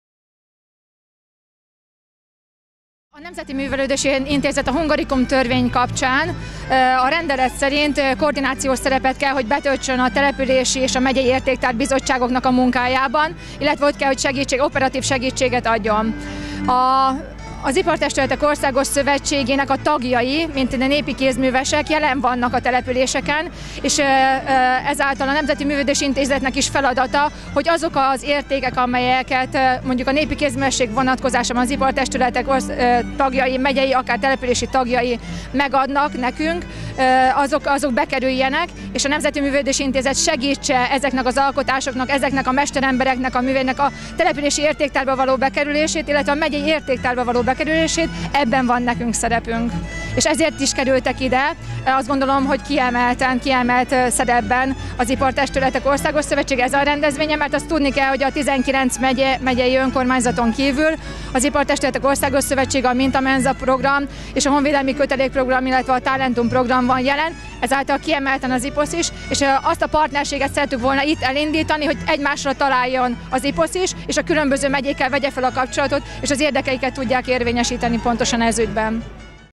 Závogyán Magdolna - interjú / Magyar Értékek 2013.09.14. NMI-IPOSZ
Závogyán Magdolna, a Nemzeti Művelődési Intézet főigazgatója
Magyar Értékek - Hagyományteremtő program a Szent István téren 2013. szeptember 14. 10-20 óráig